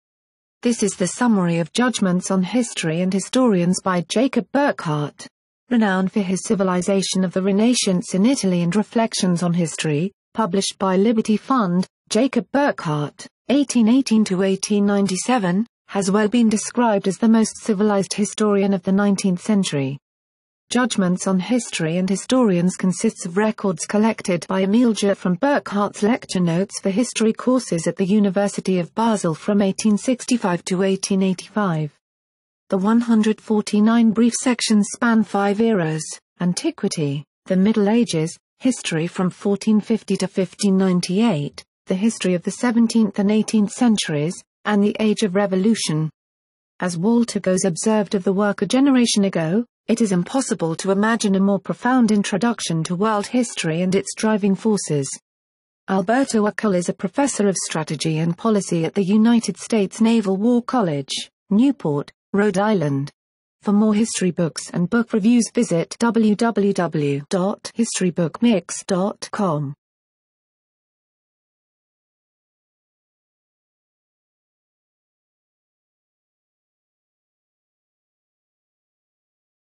History Book Review: Judgments on History and Historians by Jacob Burckhardt
This is the summary of Judgments on History and Historians by Jacob Burckhardt.